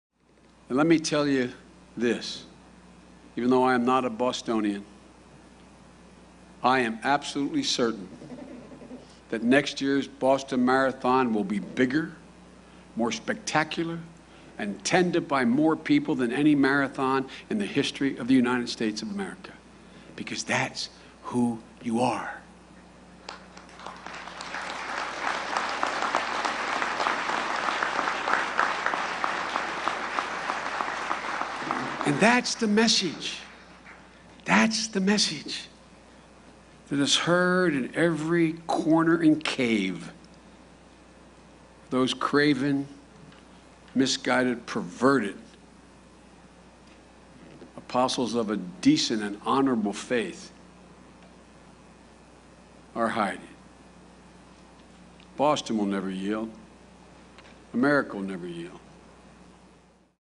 2014 Boston Marathon will be
Vice President Biden, speaking at the memorial service of a MIT police officer who was killed in pursuit of the Boston bombing suspects, predicted that